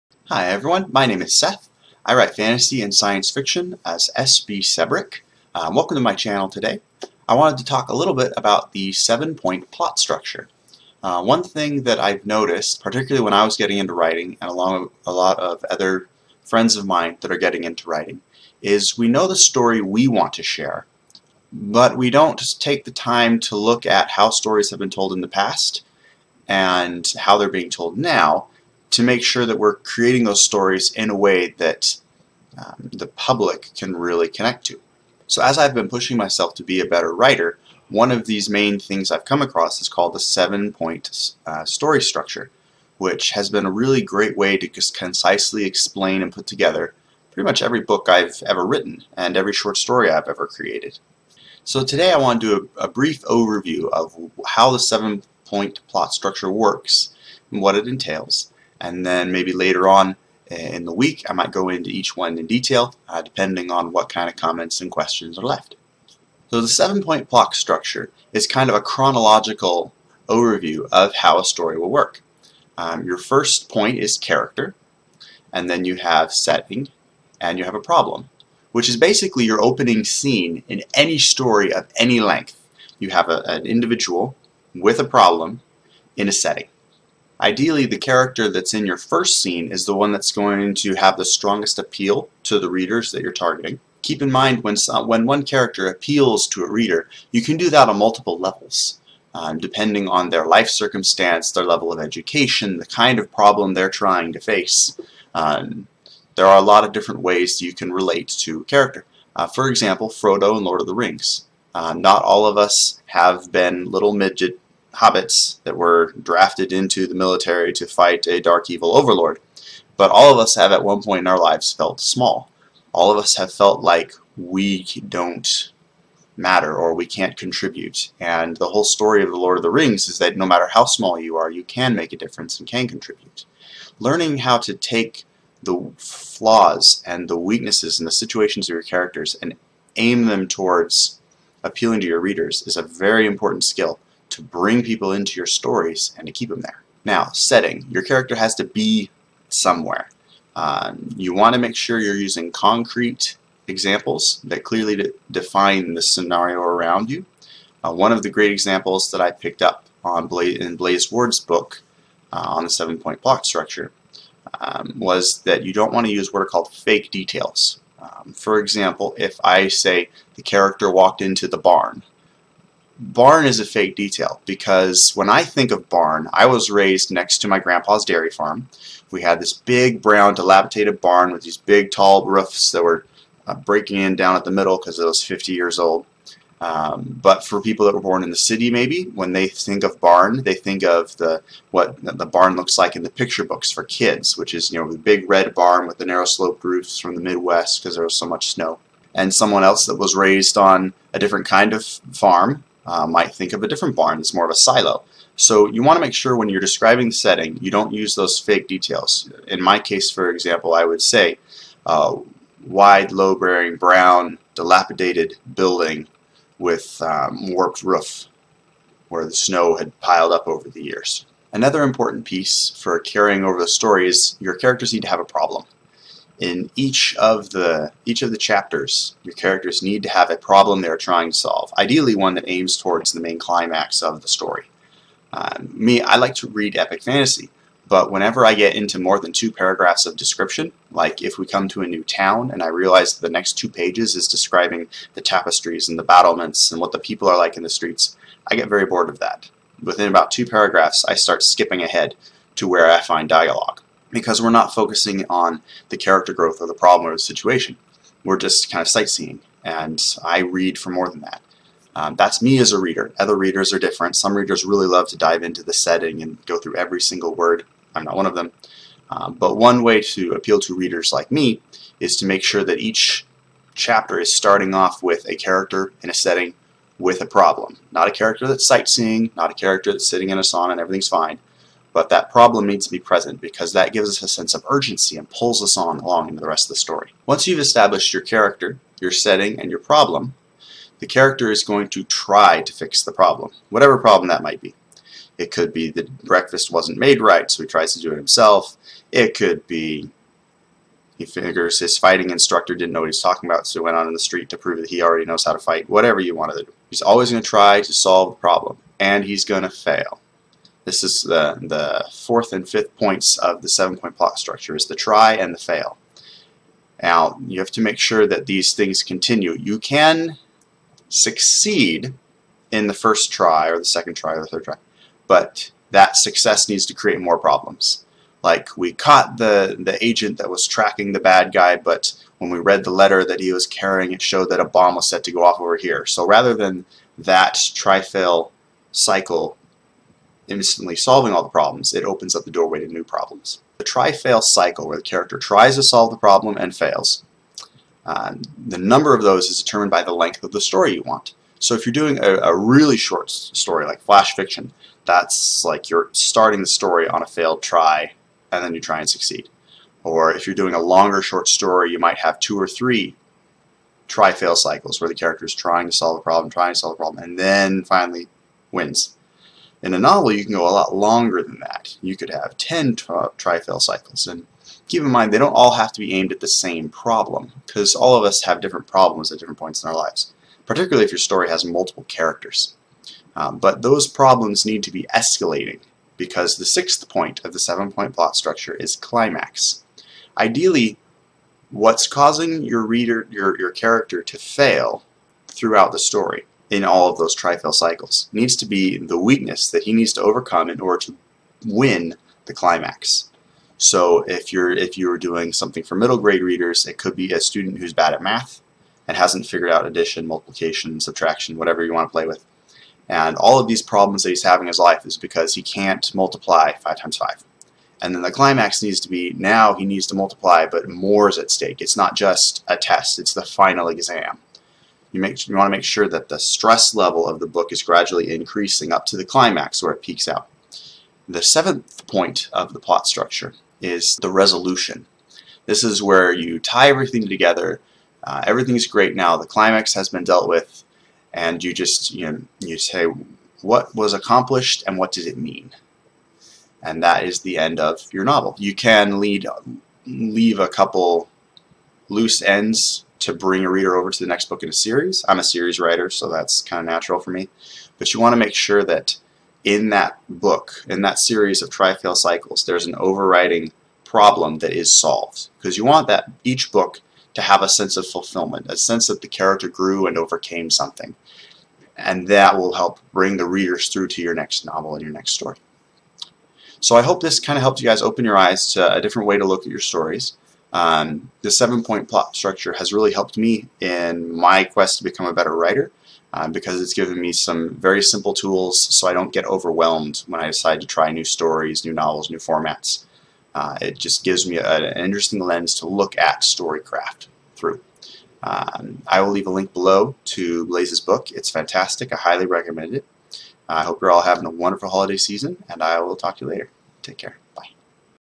The Seven Point Plot Structure
A brief overview of how this plot structure can help writers improve on their stories and better connect with their readers. These are
powerful writing tools from Blaze Ward's novel, "The Beginning Professional Storyteller." Check out the link below for more.

If you want to learn more, here are a few ways to keep in touch.